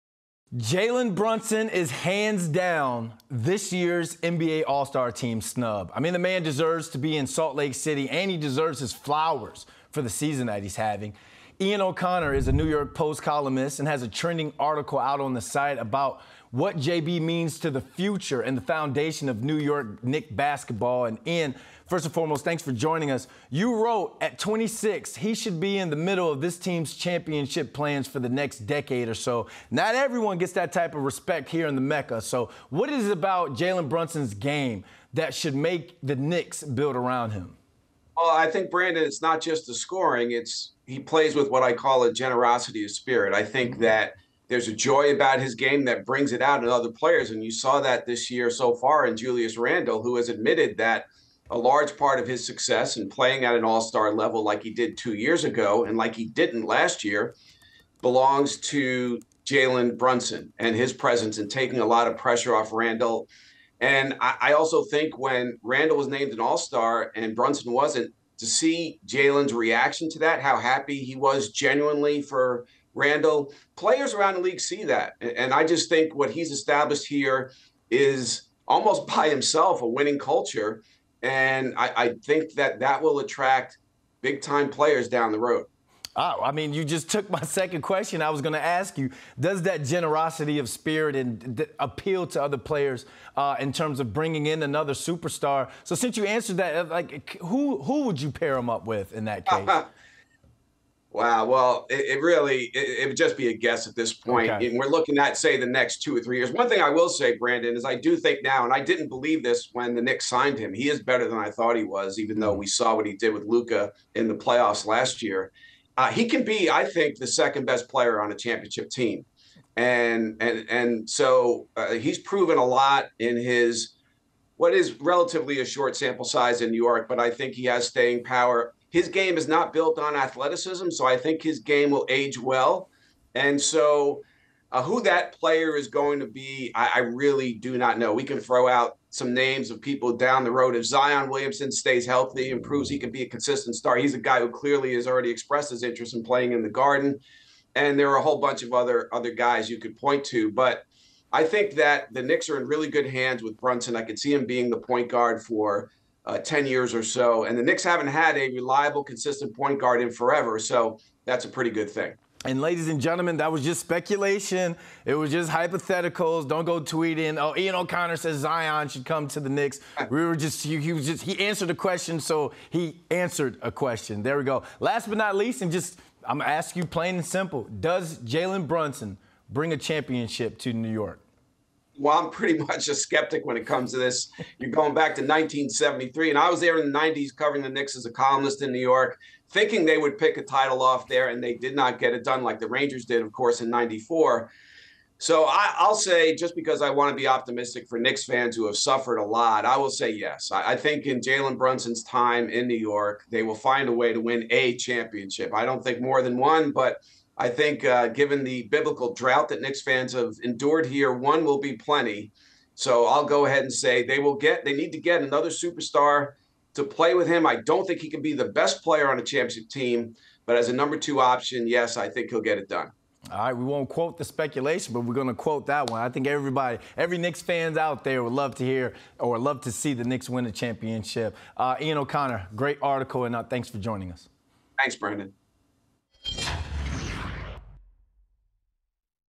Knicks’ Jalen Brunson snubbed from NBA All-Star team | SNY
The Knicks’ Jalen Brunson got snubbed from NBA All-Star team, despite his top-notch game and winning presence. NY Post sports columnist Ian O’Connor joins Brandon London to discuss Jalen’s performance this year and what he means to the future of the Knicks.